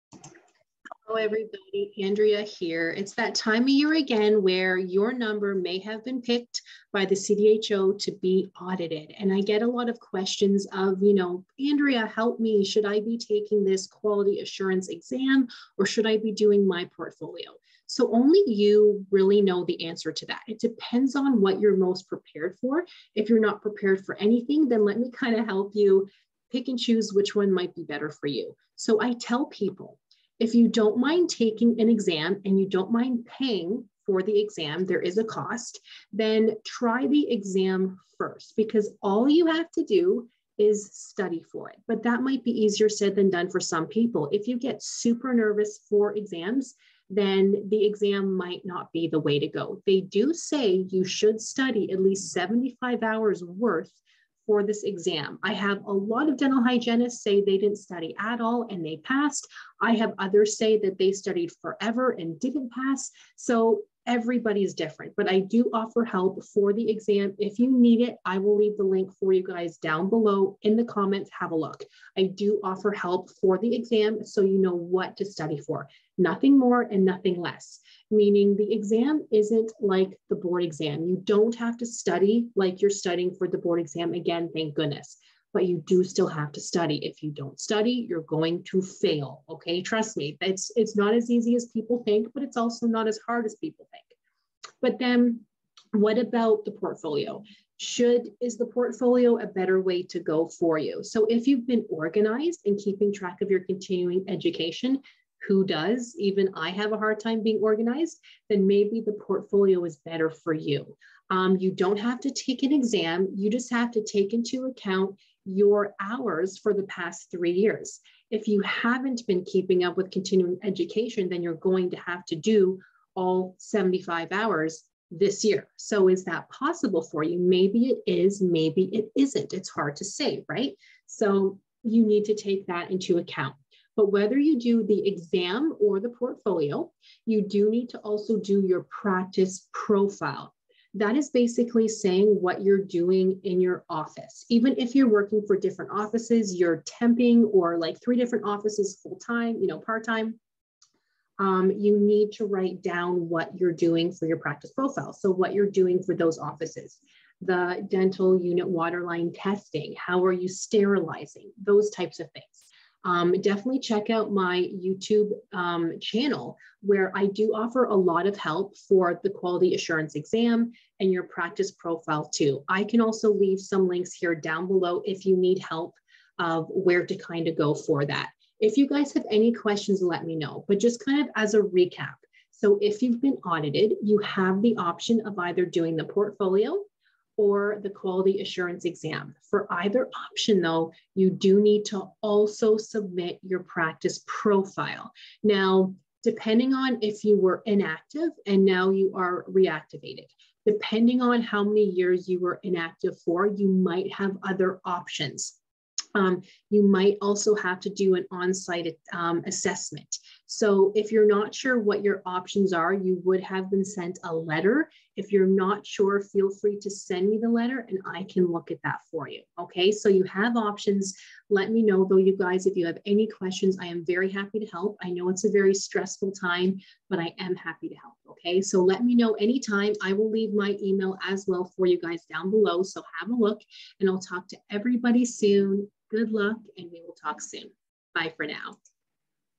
Quality Assurance Exam, Practice Profile, and Portfolio Options for RDH's
Are you an RDH in Ontario who has been audited for the quality assurance program with the CDHO?  You have the options of a portfolio, quality assurance exam, and don't forget your practice profile!